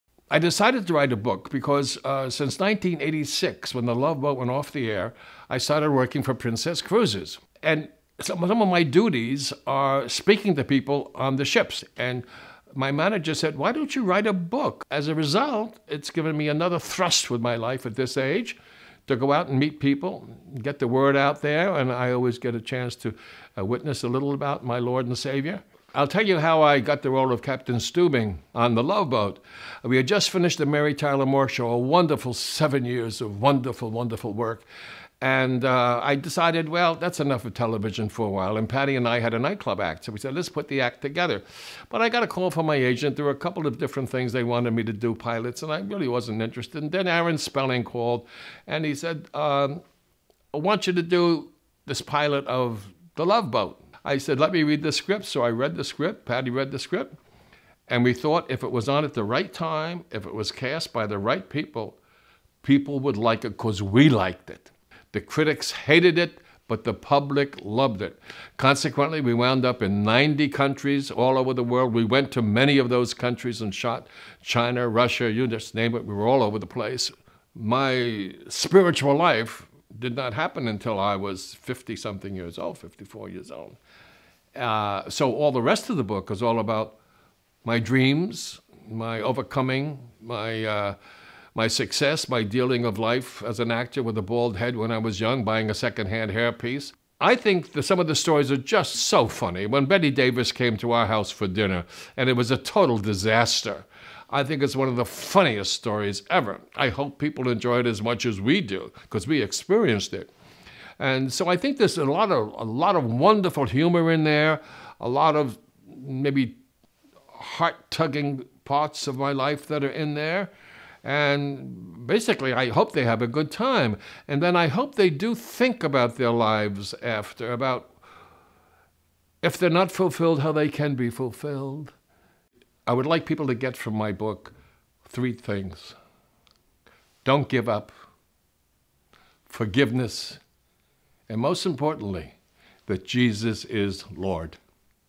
This Is Your Captain Speaking by Gavin Macleod
This is your Captain Speaking by Gavin Macleod book trailer.  A book published by Thomas Nelson.